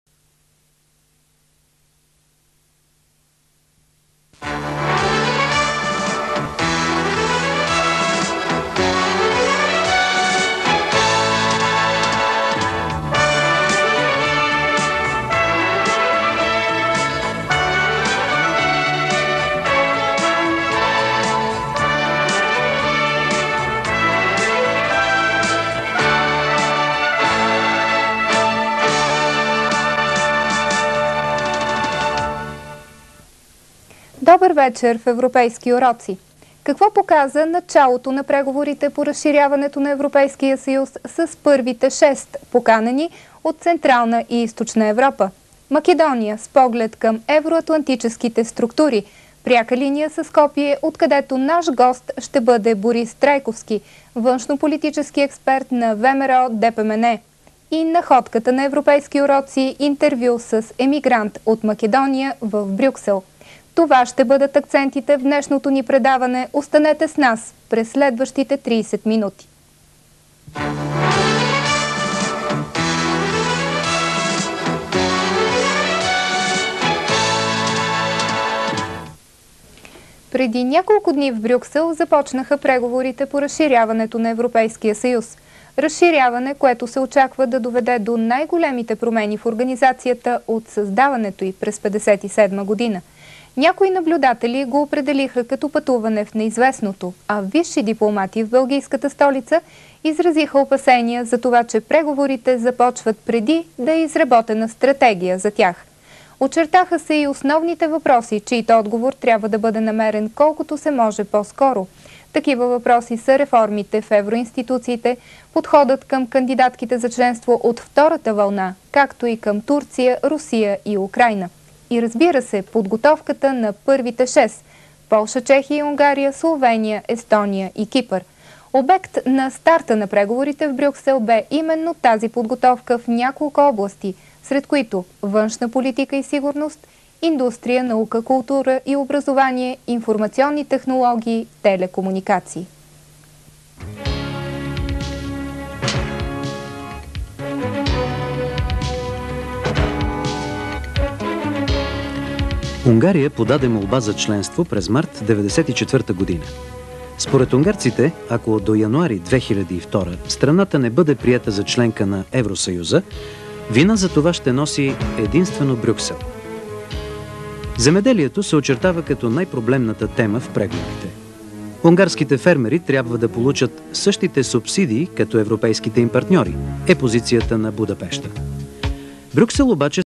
Liudmila Stanoeva- Europa
Liudmila Stanoeva, author and presenter of political tv show on NATO and EU